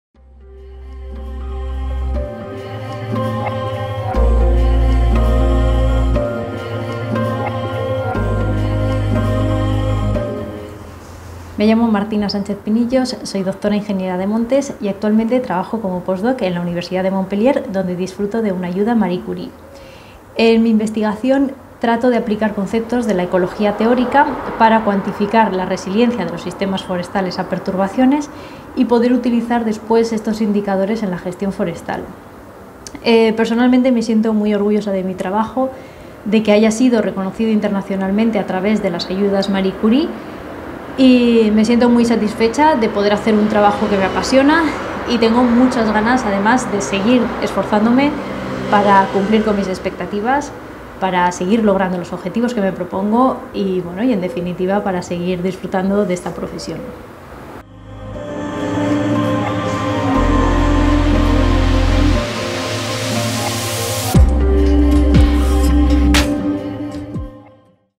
Saludo de Martina Sánchez Pinillos en el Día Internacional de la Mujer 2021
Martina es Dra Ingeniera de Montes y actualmente tiene una beca Marie Curie en la Universidad de Montpellier